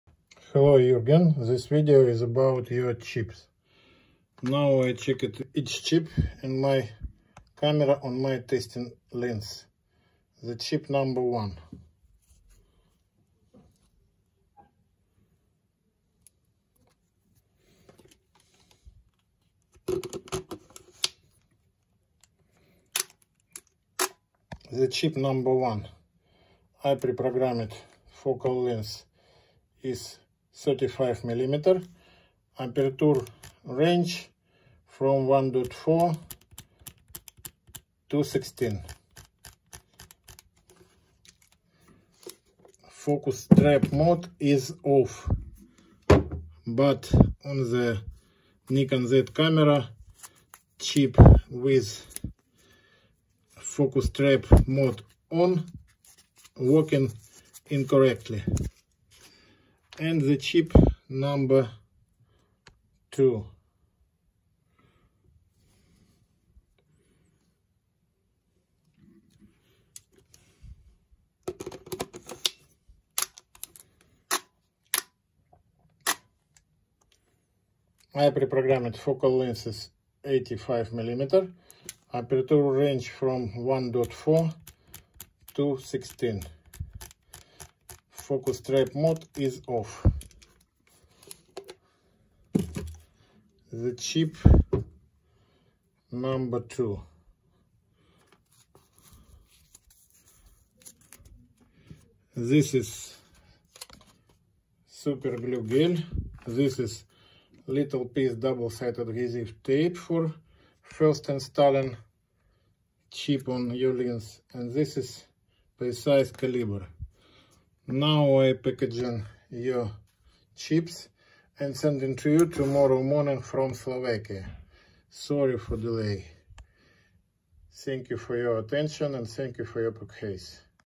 For Juergen P.
Nikon AF PROGRAMMABLE dandelion chip. 2 pcs.
1. For  Nikkor 85mm 1.4 AIS
2. For Nikkor 35mm 1.4 AIS